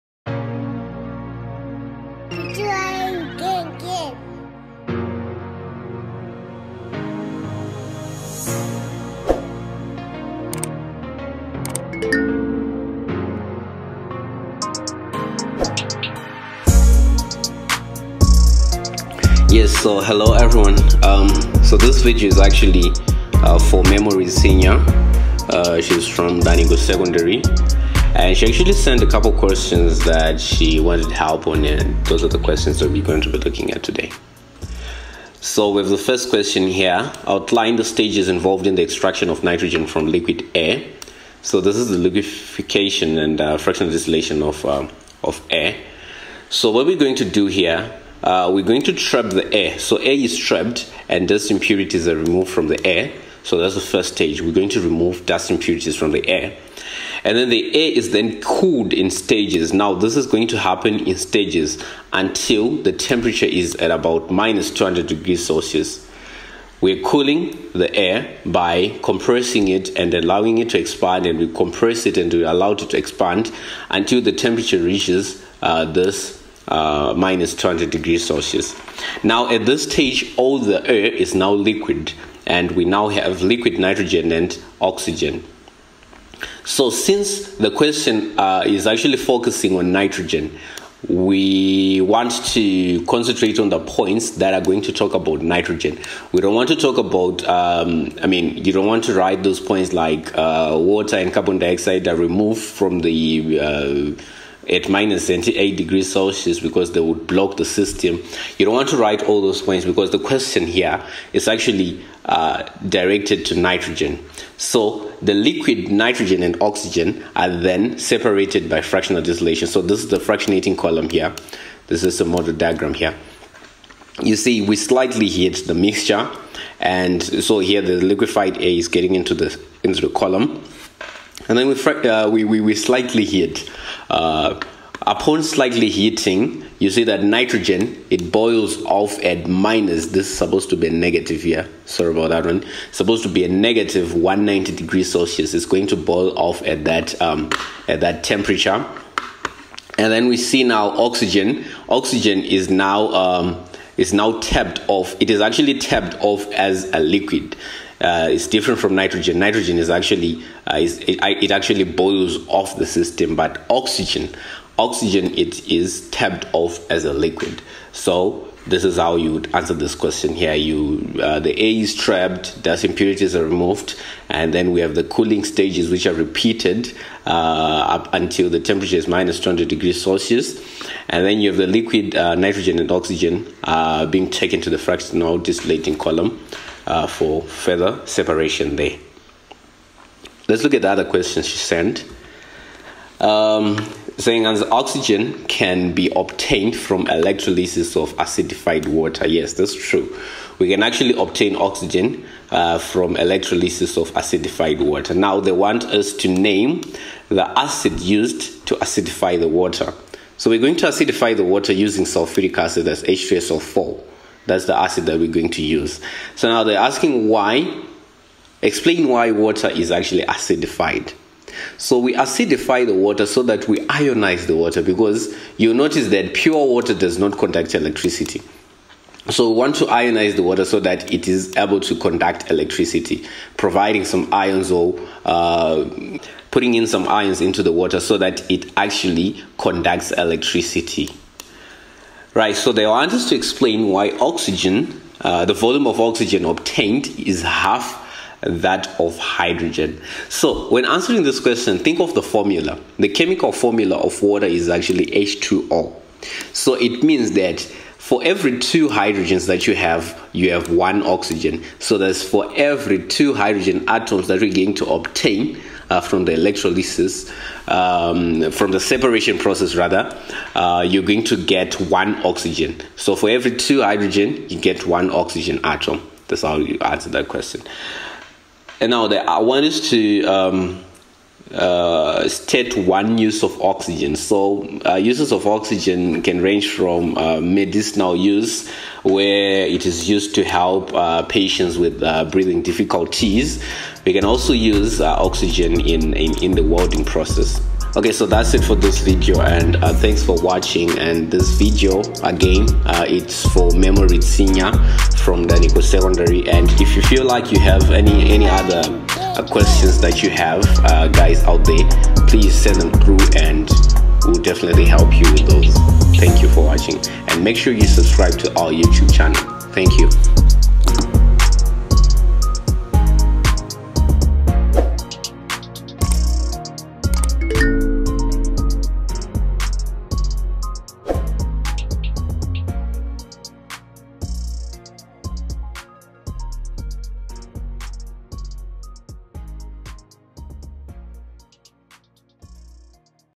ZIMSEC Combined Science November 2019 Paper 2 Revision
This video walks you through the possible solutions of a typical ZIMSEC Combined Science questions sent by Memory Tsinya from Danhiko Secondary School in Zimbabwe.

Enjoy!! Learn!!

- ZIMSEC Combined Science November 2019 Paper 1 Revision

- ZIMSEC Mathematics Paper 1 2019 O Level